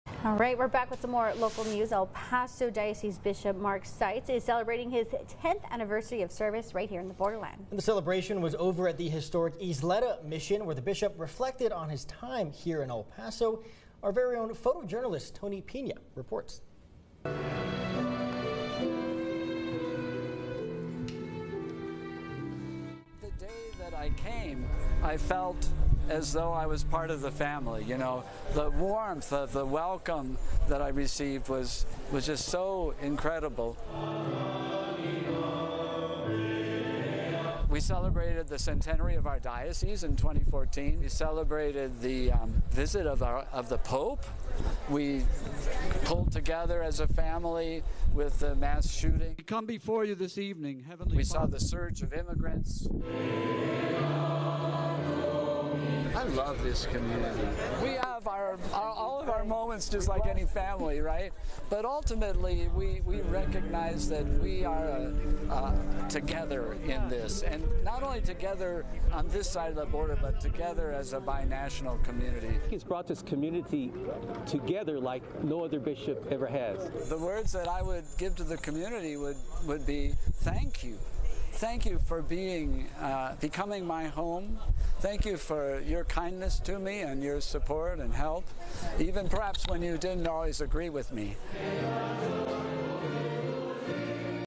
Seitz to mark 10 years as bishop of El Paso with celebration at Ysleta Mission
El Paso Bishop Mark Seitz is celebrating his 10th anniversary as bishop.